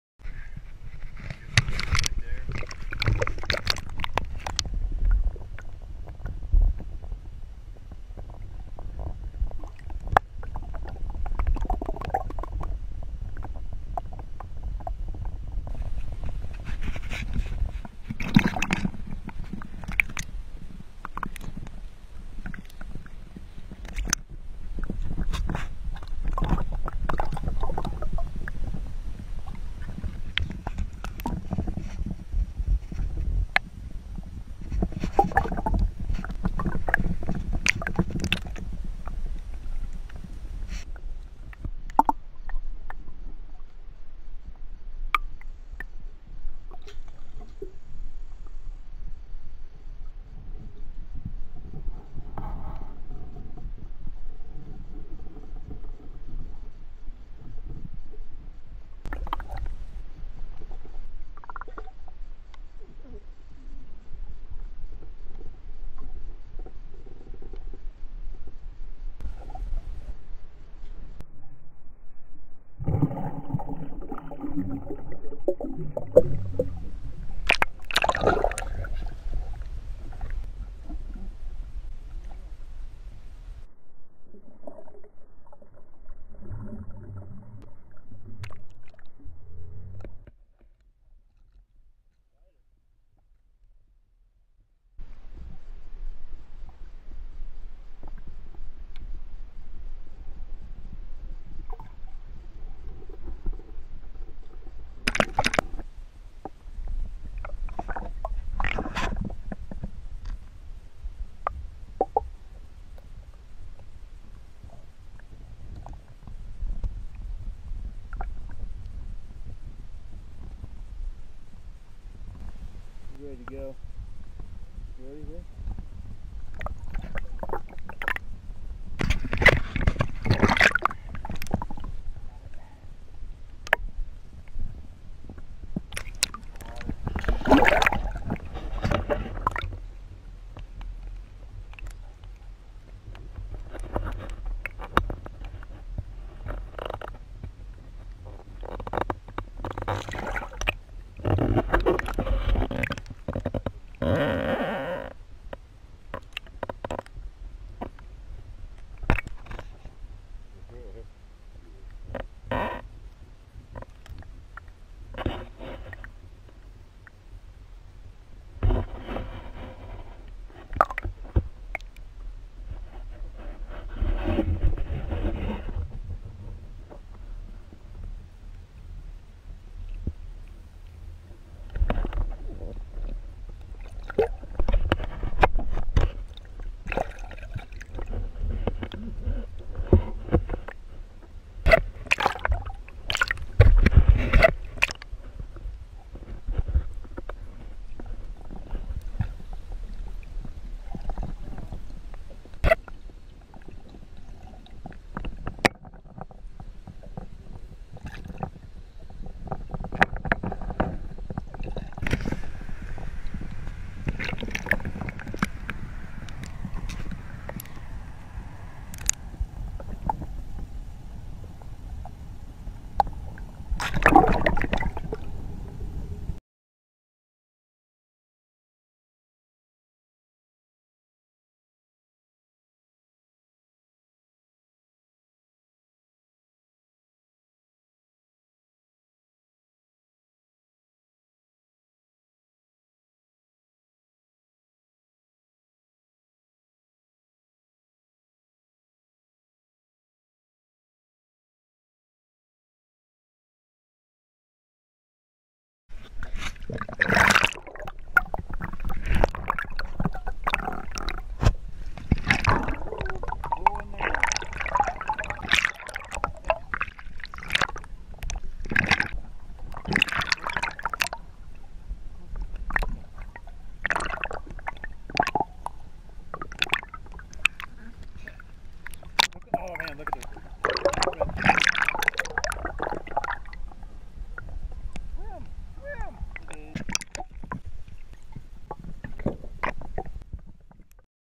All under water bass releases vol 1
Here’s a video to zen out to of under water bass releases captured through-out the years.